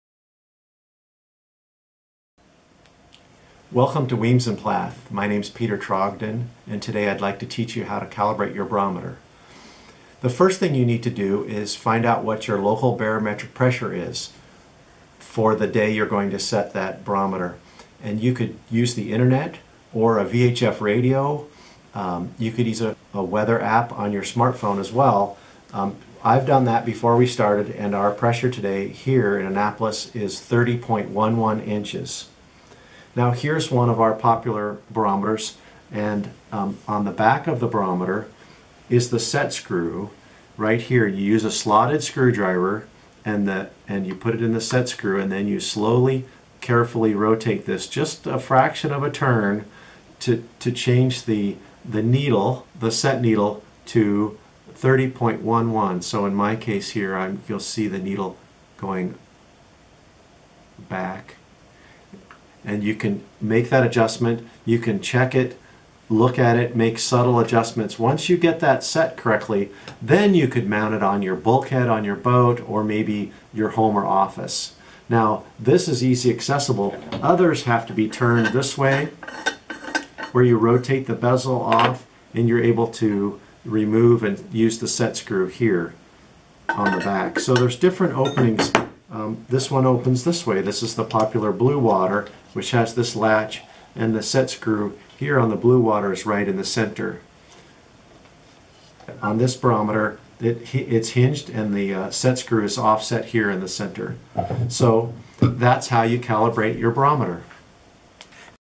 Calibrating Your Barometer
Our president at Weems & Plath, Peter Trogdon, takes time to show how to calibrate a few of our barometers. This video is helpful in showing how to determine what the pressure of the barometer should be set to and how to adjust it.